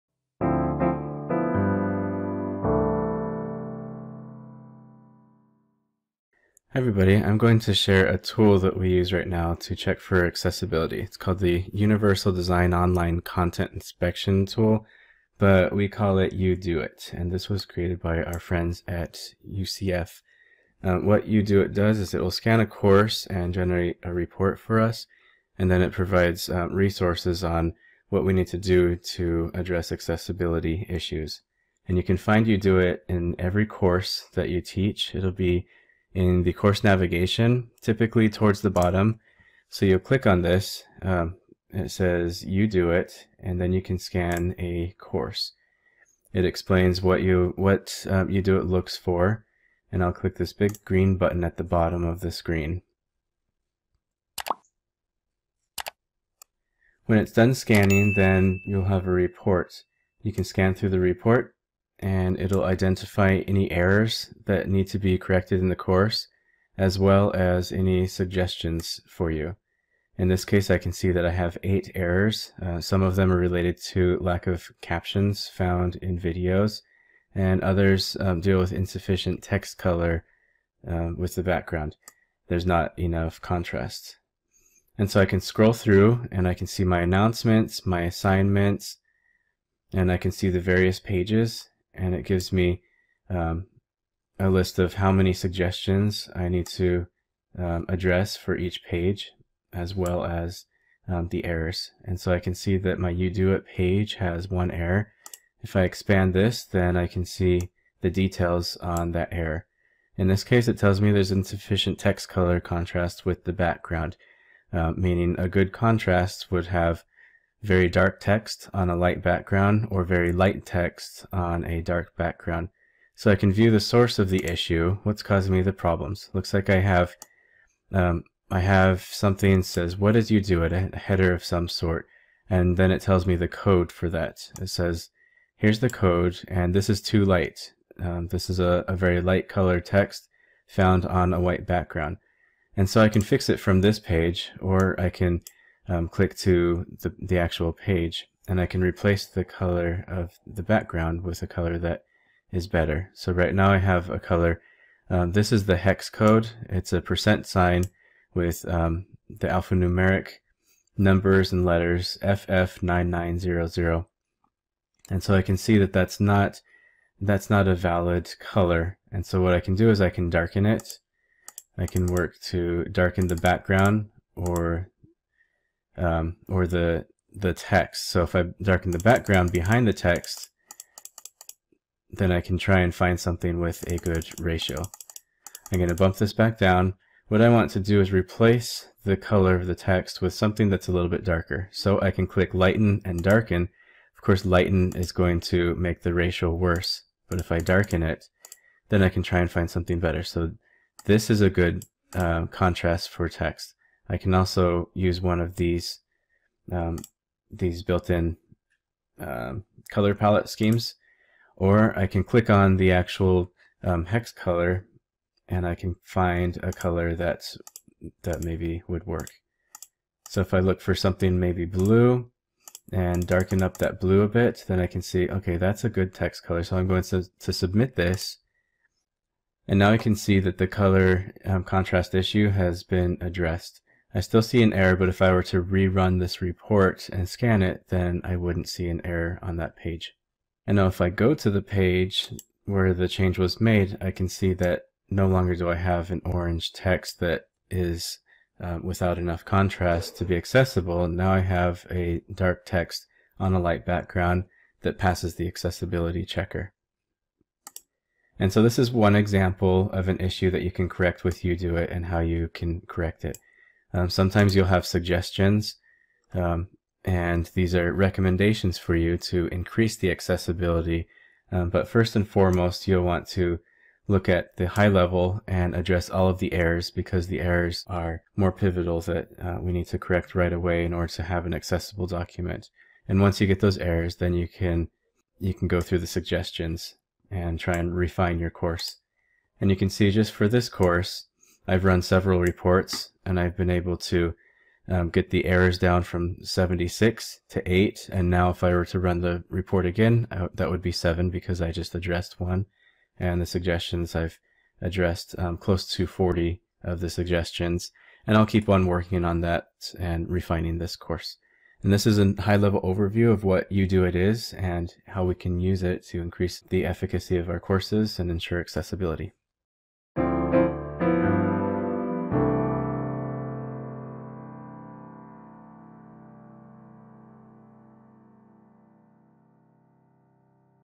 UDOIT Overview
The Universal Design Online Content Inspection Tool, or UDOIT (pronounced, “You Do It”) is a course accessibility tool that allow us to scan our course content and generate a detailed report of accessibility issues, errors and recommendations.

UDOIT identifies “errors” and provides “suggestions” in the following areas of your course:

Announcements
Assignments
Discussions
Files (i.e. html files or documents)
Pages
Syllabus
Module URLs
It will ensure that that documents and course materials adhere to the 5 Principles for creating accessible documents.